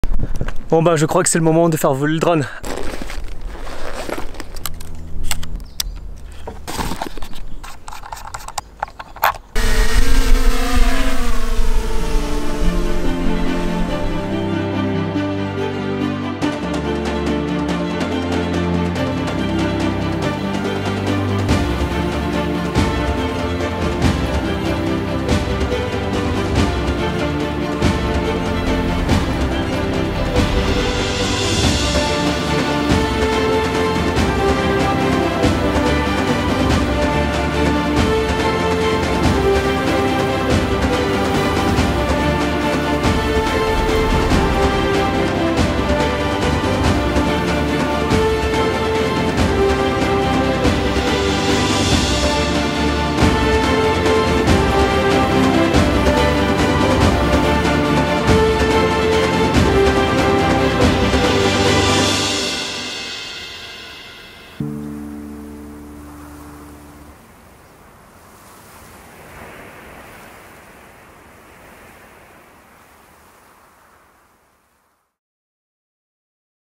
Martinique Drone 4k | DJI Mavic Air 2s
Premier test de mon DJI Mavic Air 2S entre la montagne pelée et les pitons du carbet qui étaient bien visibles ce jour-là !

Ce drone est juste incroyable.

Pour découvrir les meilleurs endroits en Martinique, adonne-toi et rejoins-moi sur mes réseaux :

Alexandre